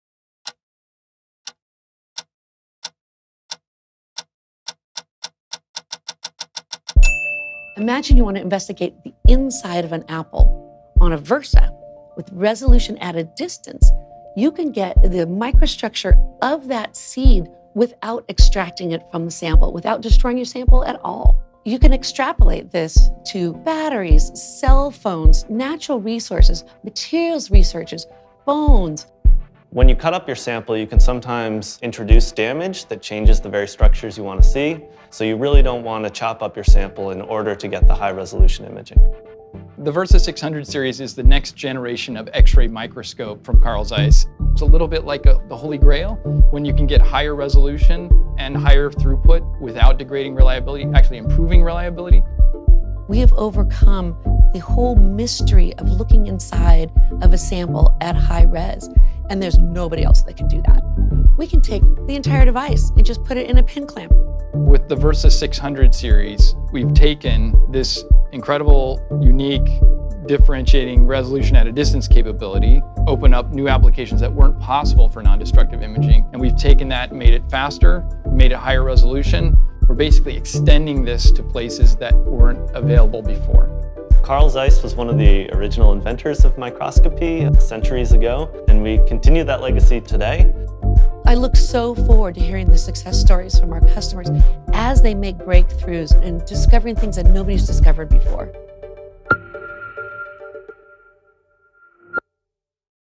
ZEISS Xradia 610 and 620 Versa
A new chapter in non-destructive imaging of intact samples at sub-micron scale resolution.